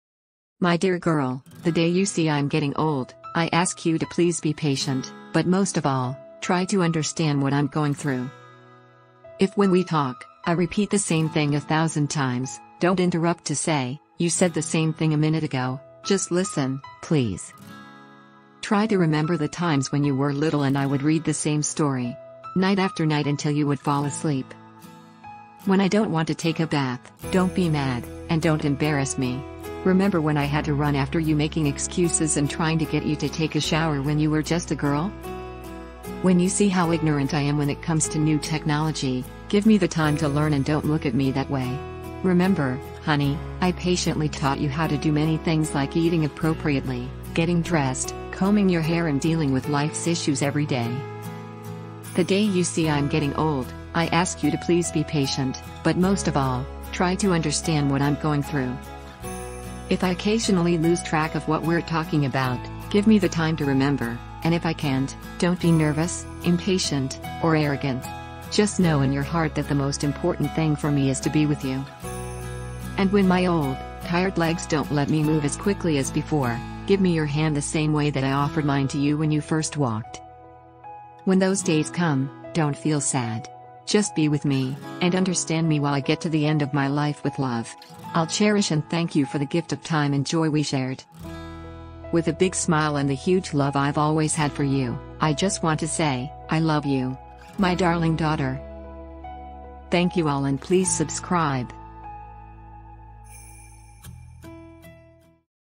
Parents Reveal Secrets to Raising Happier Kids: You Won't Believe What Happened Next!
This video is about mom and dad getting old.
Mom and Dad look Back On life